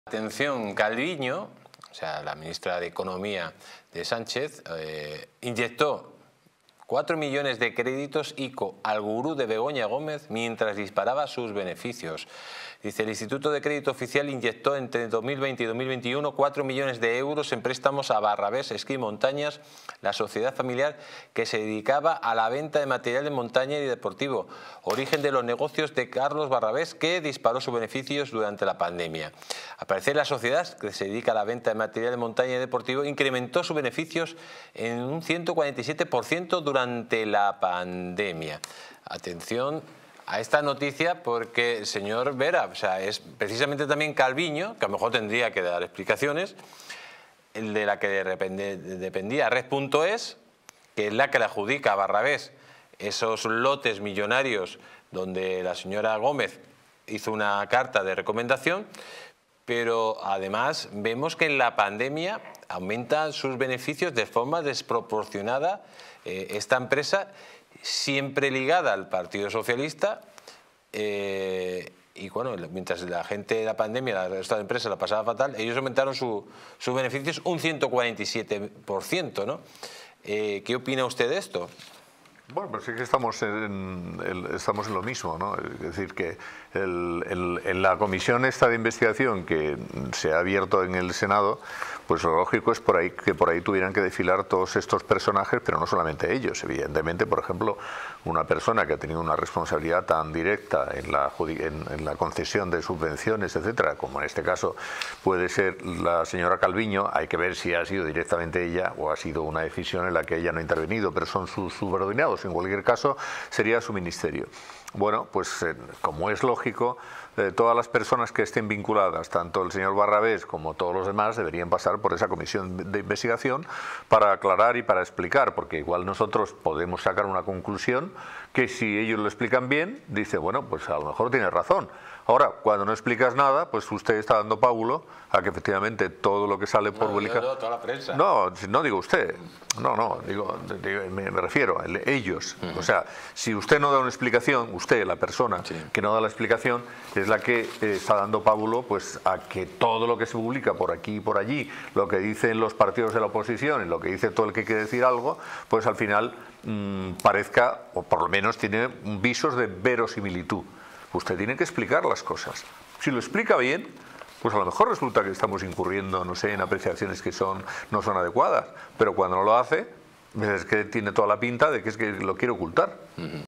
Calviño inyectó 4 millones de créditos ICO al gurú de Begoña Gómez mientras disparaba sus beneficios
En este vídeo, descubre cómo Calviño inyectó 4 millones de créditos ICO al gurú de Begoña Gómez, mientras sus beneficios aumentaban. ¡Mantente informado con Distrito TV!distritotv